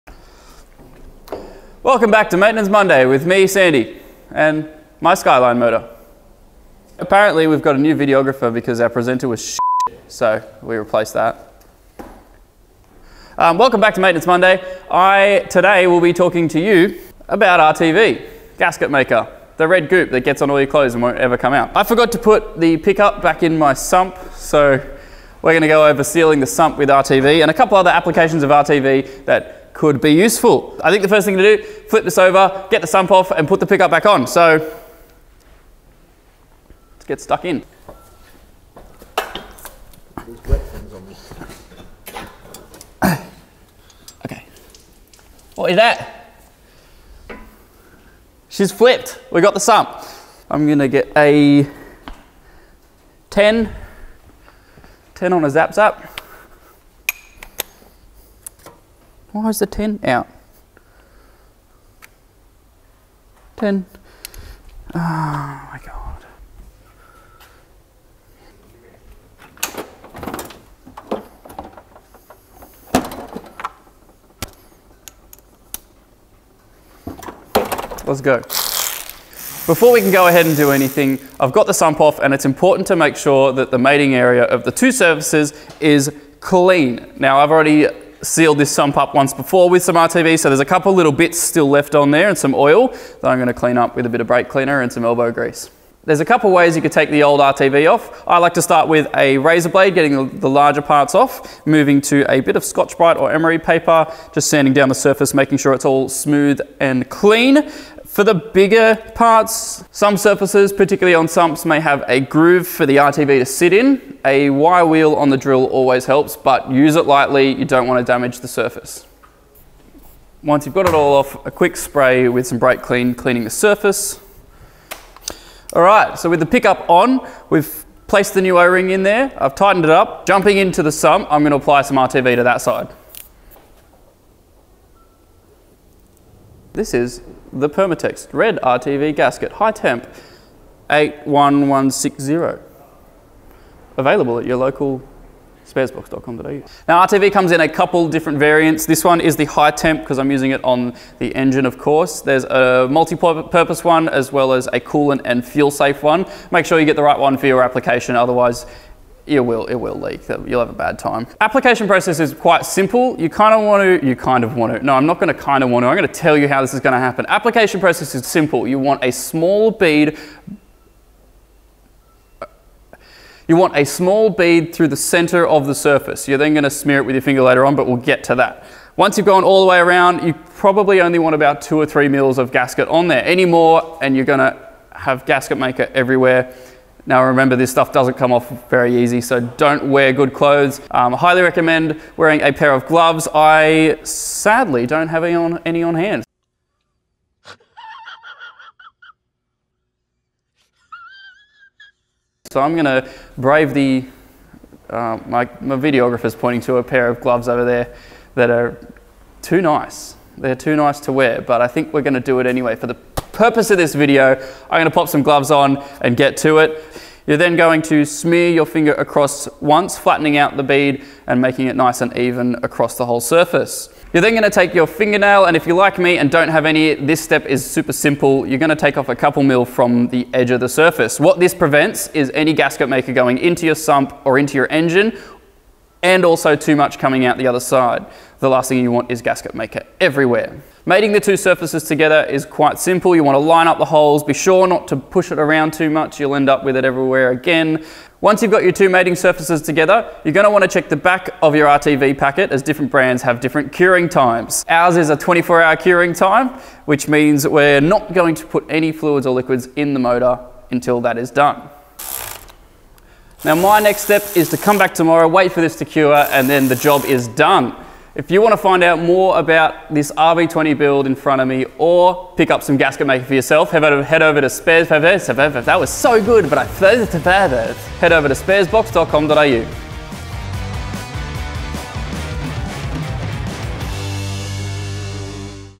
Maintenance Monday - How To Seal Your Sump with RTV!
Sealing your sump and using RTV doesn't need to be scary, if our slightly silly videographer can do it - you can too!